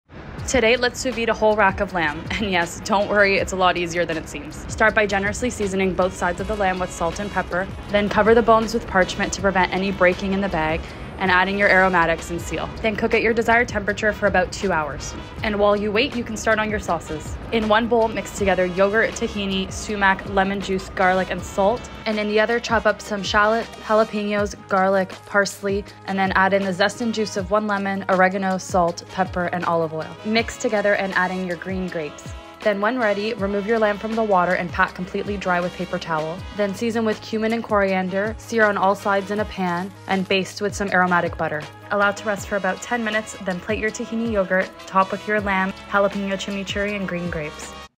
Sous Vide Rack of Lamb with Sam Medeiros and the Anova Precision® Cooker
Sam Medeiros gets it, cooking a whole rack of anything at home can be extremely daunting. Are you overcooking it/undercooking it? Is it smoking out your kitchen? Will it still be moist when you’re done cooking it or will it end up being dry? It’s a lot of variables to worry about. A way to avoid all the worry and to make it as straightforward and simple as possible is to cook the whole rack sous vide. By cooking the lamb rack sous vide, you can ensure the exact temperature and cook time, while keeping it moist and tender (since it cooks in its own juices). It’s a pretty foolproof way if you’re not used to cooking racks of lamb whole, and limits your active cooking time by a lot. Set your Precision Cooker, put your lamb in the water, make a few sauces to go with it, and enjoy the rest of your downtime before you finish the lamb quickly in the pan. Cooking Temperature Chart Rare: 115°F (46°C) to 124°F (51°C) Medium Rare: 125°F (52°C) to 134°F (57°C) Medium: 135°F (57°C) to 144°F (62°C) Medium Well: 145°F (63°C) to 154°F (67°C) Well Done: 155°F (68°C) and up ** Note: Cook for 2 1/2 hours max if under 130°F/54°C